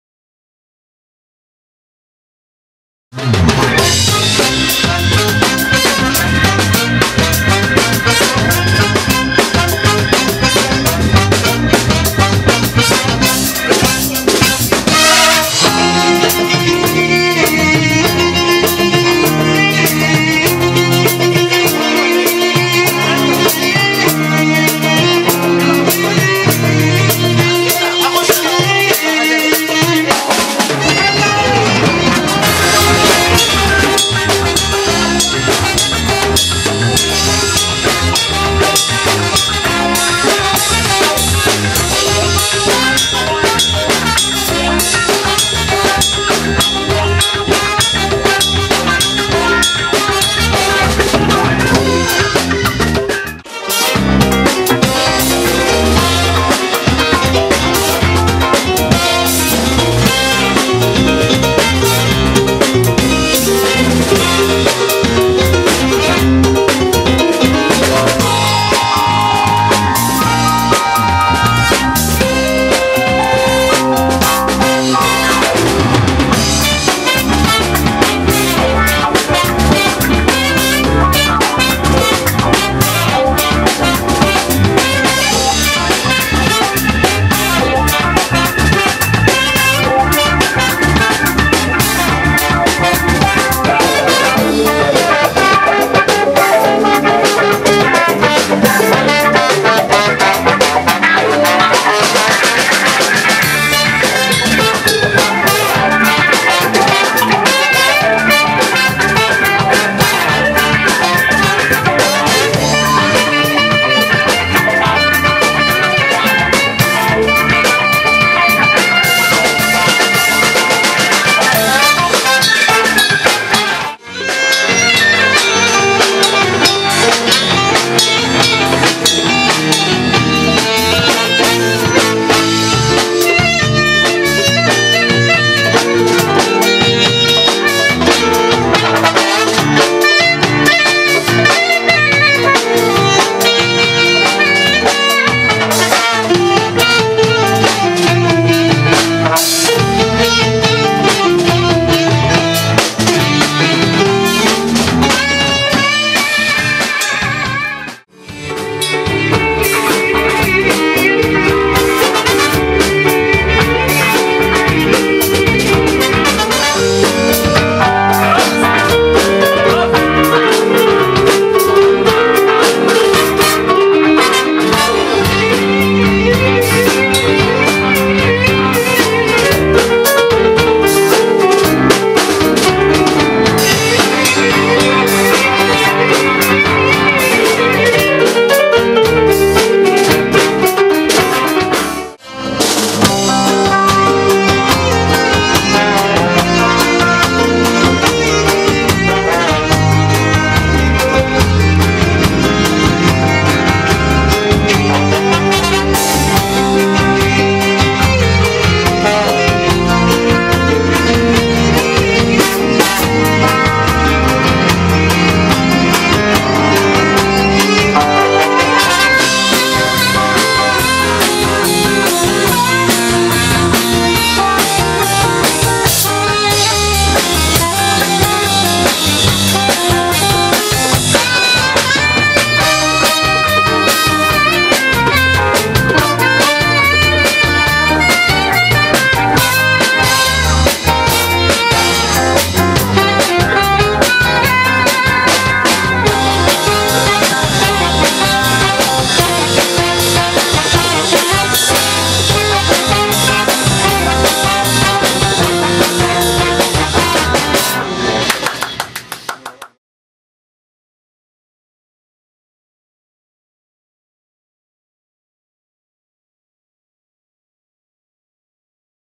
Groove Project Arabia - Promo Vol.1
Members Band:    Mohamed El.Banhawy (Piano , Keyboard)
Mutaz Dawoud (Saxophone)
Nezar Masoud (Bass guitar)
Mohamed maani (Keyboard)
Ahmed disoky (Lead Guitar)
Amr Hamdy (Drums)
Amr El.Moursy (Trumbone)
Mohsen (Trumpet)
Hany (Violin)
Taher Saleh (Sound Eng.)